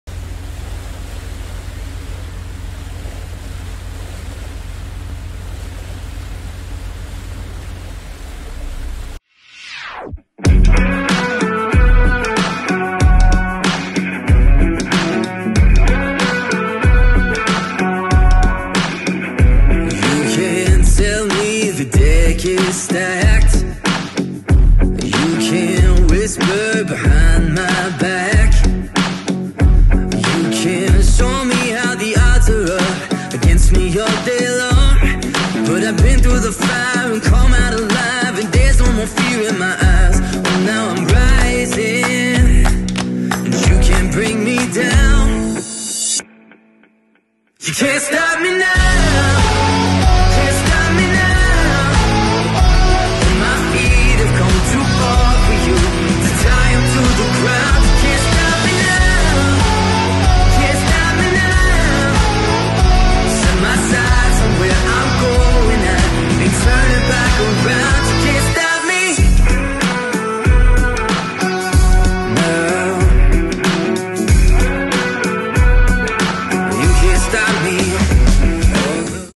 Jet skis at Calshot
Jet skis playing around at Calshot Castle, Hampshire UK.

Shot on the DJI Mini 2, edited on Adobe Rush